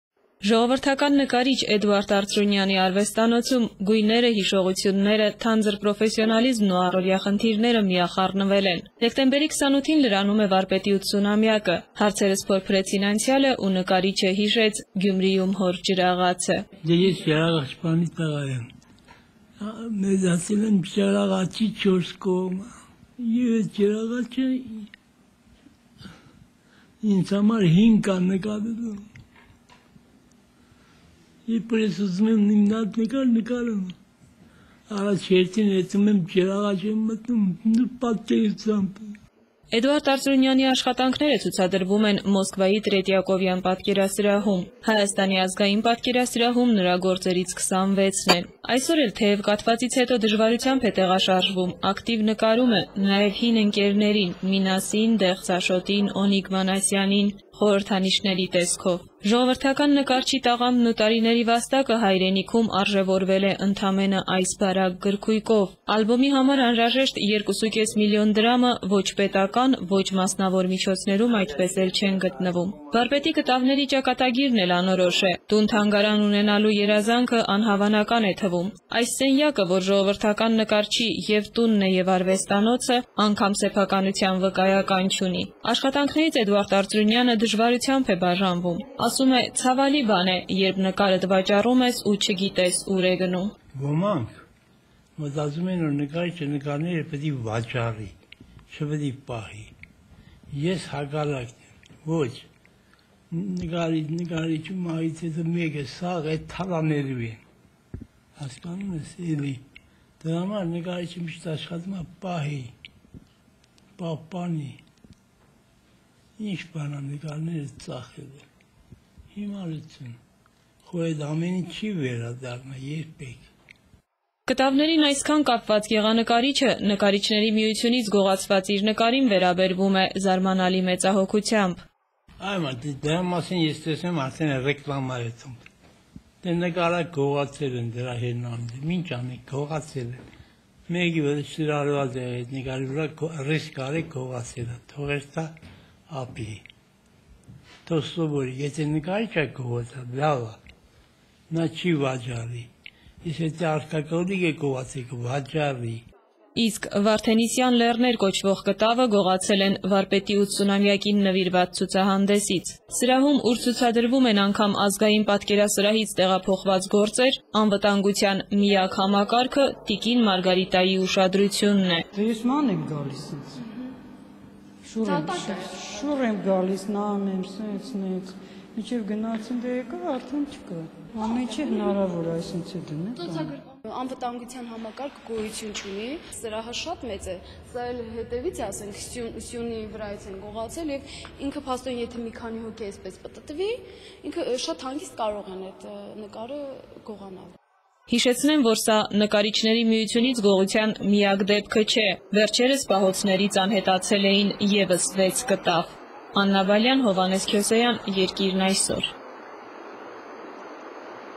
Տաք ու սառը գույների մեջ
Ժողովրդական նկարիչ Էդուարդ Արծրունյանի արվեստանոցում գույները, հիշողությունները, թանձր պրոֆեսիոնալիզմն ու առօրյա խնդիրները միախառնվել են: Դեկտեմբերի 28-ին լրանում է Վարպետի 80-ամյակը: Ալբոմի համար անհրաժեշտ 2 ու կես միլիոն դրամը ոչ պետական, ոչ մասնավոր միջոցներում այդպես էլ չեն գտնվում: Վարպետի կտավների ճակատագիրն էլ անորոշ է. տուն-թանգարան ունենալու երազանքը անհավանական է թվում: